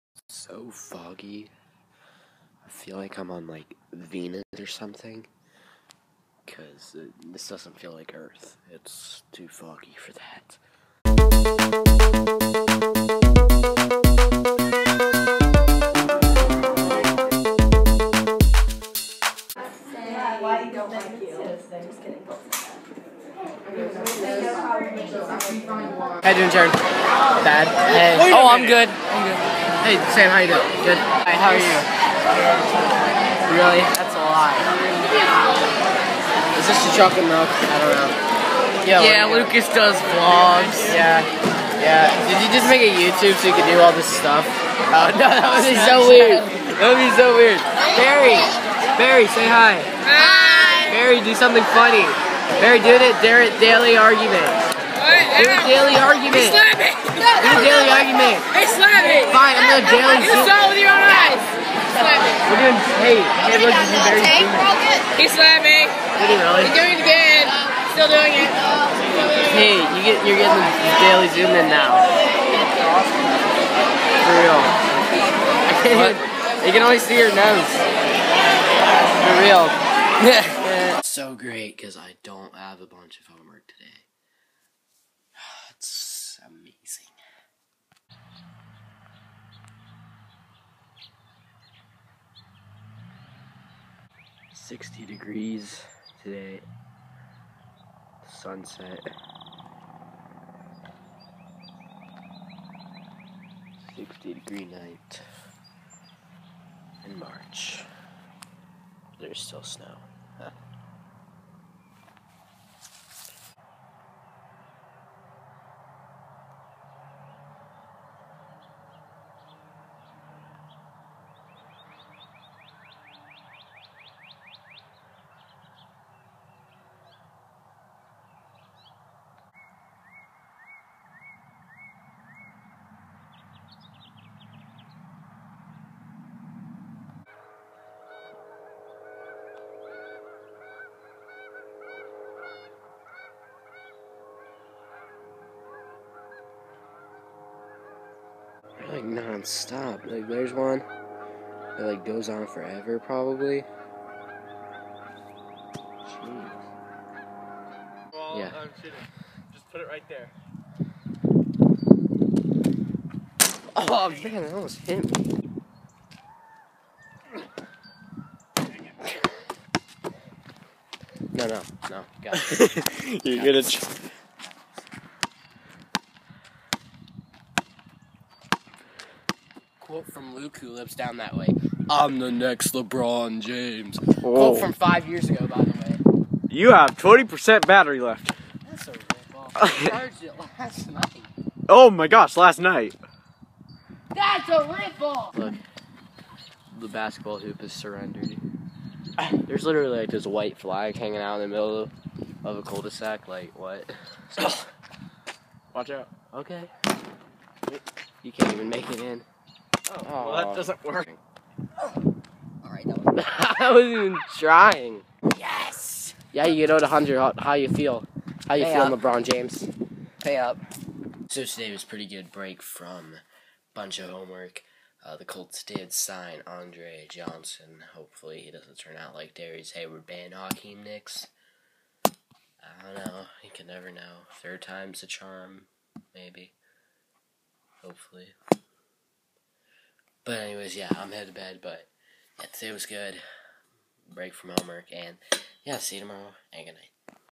Sunsets and Stuff (3/11/15 - Day 356)
It's foggy and I go to school and then it's sunny and then it's cold and then I am bad at basketball.